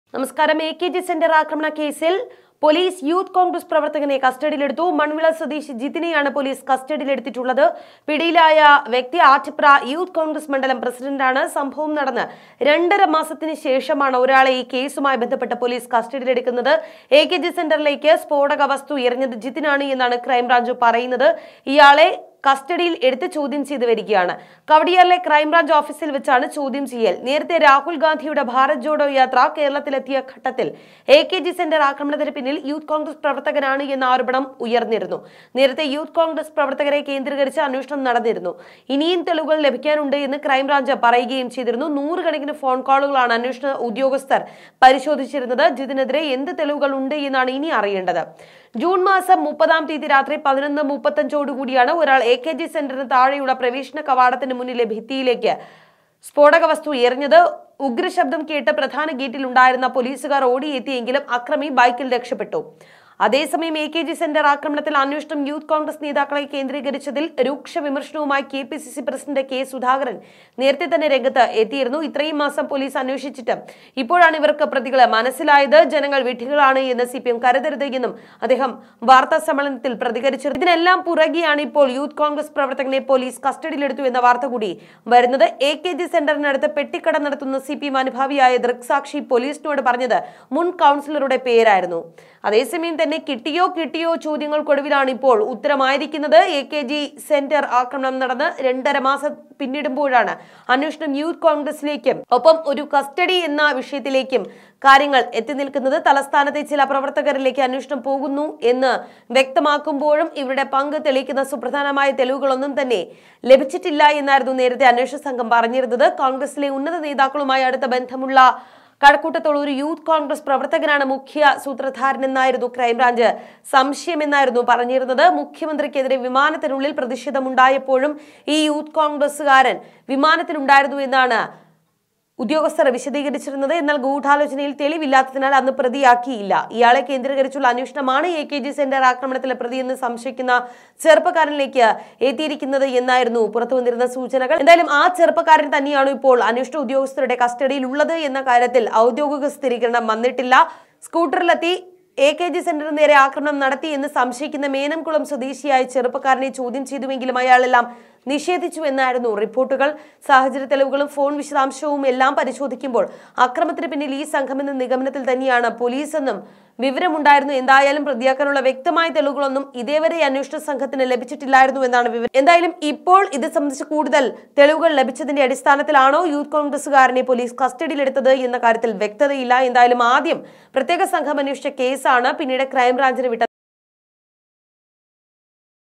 എകെജി സെന്റര്‍ ആക്രമണക്കേസ്: യൂത്ത് കോണ്‍ഗ്രസ് നേതാവ് കസ്റ്റഡിയില്‍ l akgcenter youth congress
എ കെ ജി സെന്റർ ആക്രമണ കേസിൽ യൂത്ത് കോൺഗ്രസ് പ്രവർത്തകൻ കസ്റ്റഡിയിൽ; മൺവിള സ്വദേശി ജിതിനെ കസ്റ്റഡിയിലെടുത്തു ക്രൈംബ്രാഞ്ച് ചോദ്യം ചെയ്യുന്നു; സ്‌ഫോടക വസ്തു എറിഞ്ഞത് ജിതിനെന്ന് പൊലീസ്; പിടിയിലായ ആൾ ആറ്റിപ്ര യൂത്ത് കോൺഗ്രസ് മണ്ഡലം പ്രസിഡന്റ്; സംഭവം നടന്ന് രണ്ടര മാസത്തിന് ശേഷം ആദ്യ കസ്റ്റഡി